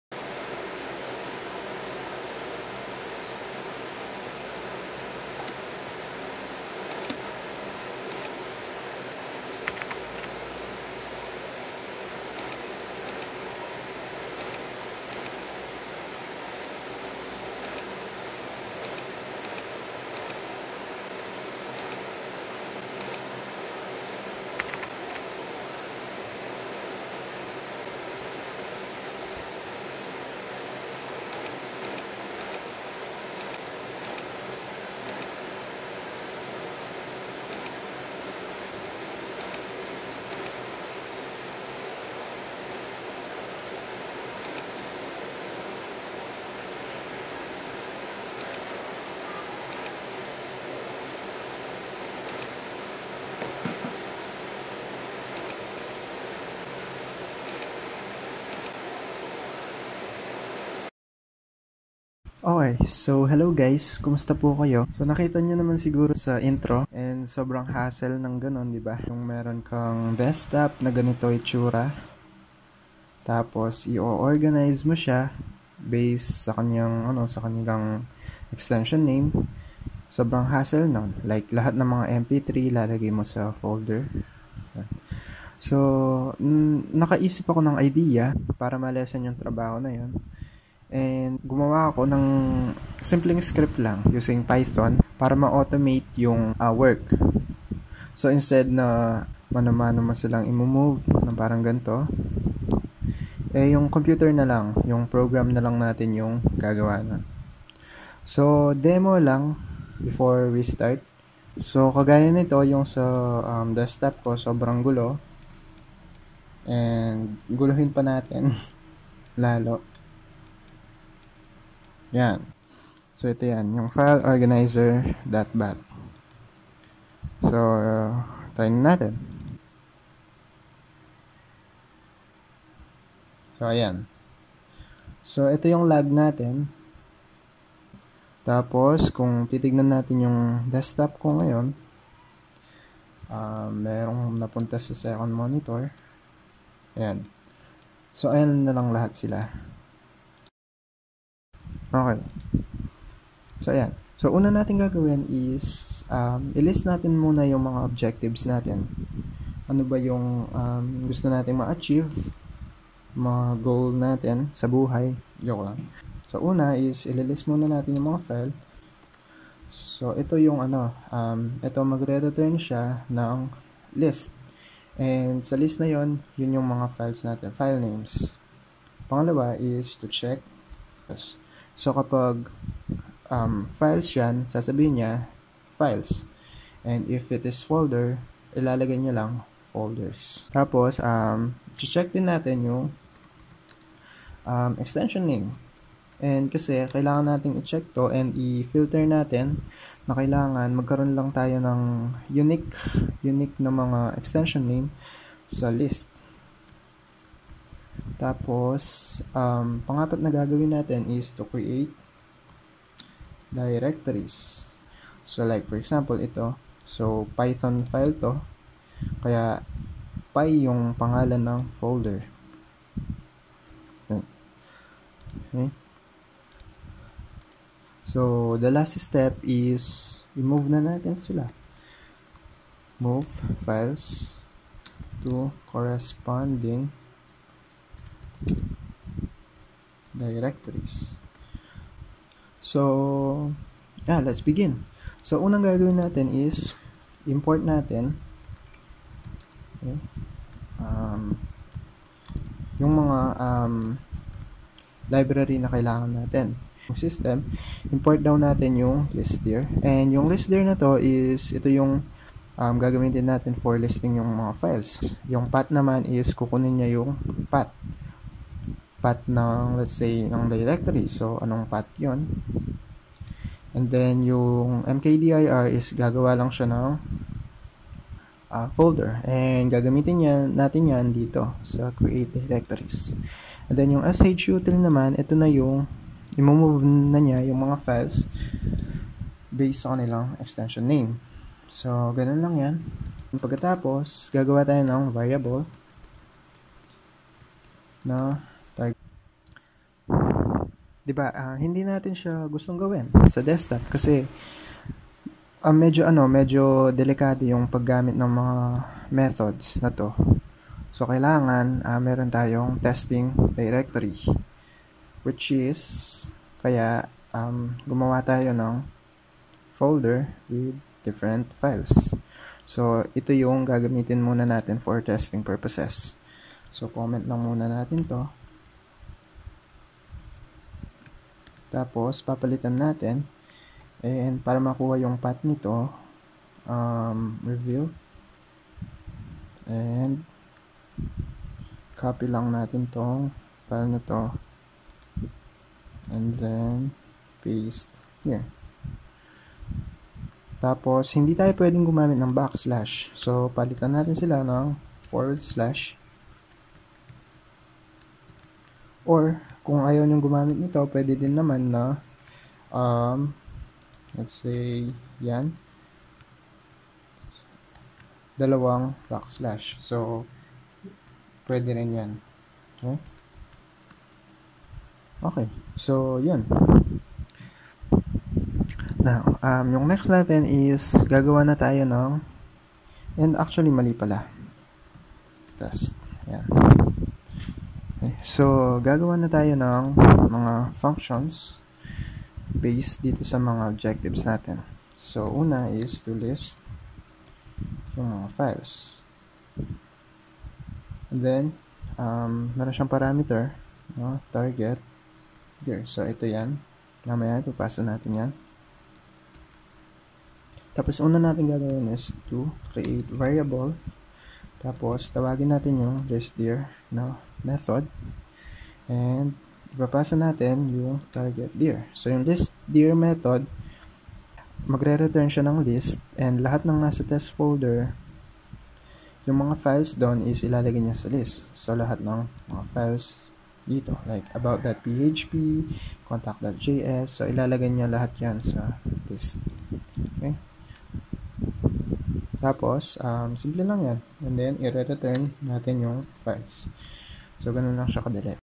Learn Python: Build File Organizer (Part 1)
Let's make learning of Python fun by building a useful project like File Organizer for your computer.

This is part 1 which is the demo of the file organizer system and the objectives of it.